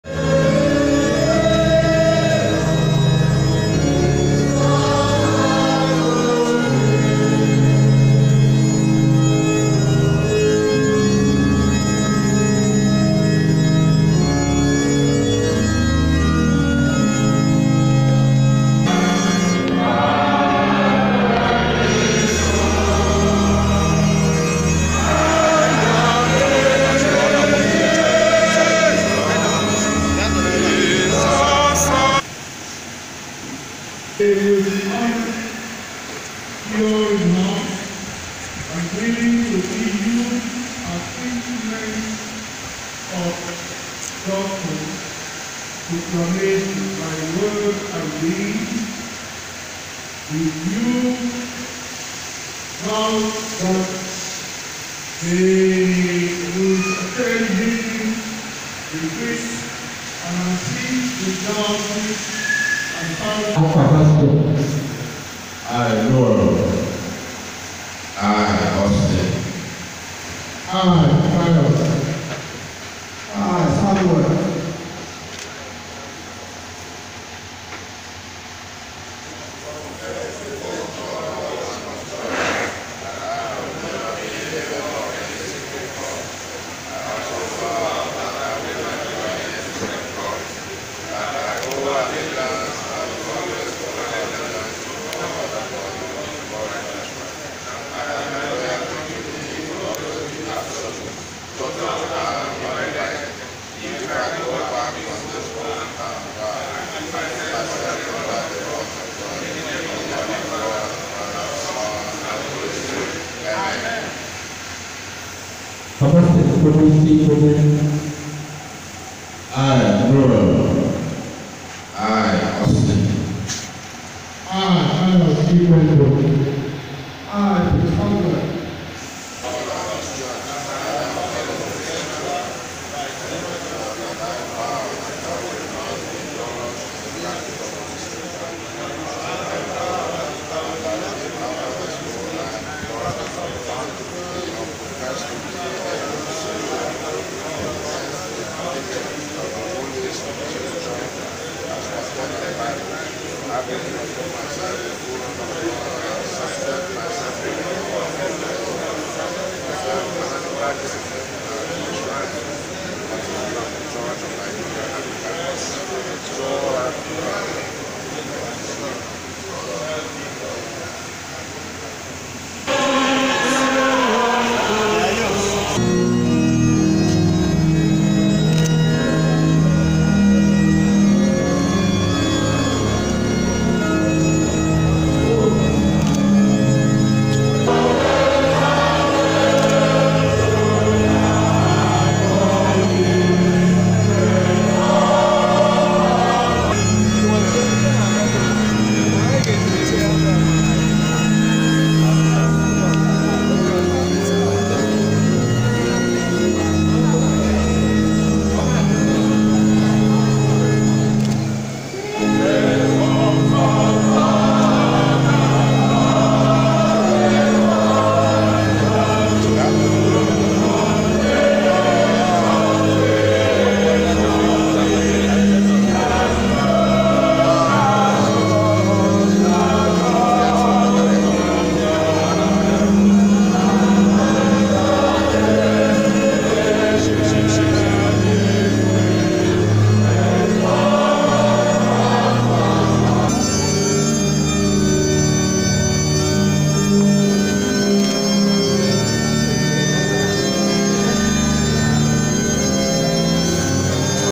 Church Service For Knighthood of Hon. Chief Austine Chukwukere (Nwachinemere). The MP, Representing Ideato North & Ideato South Federal Constituency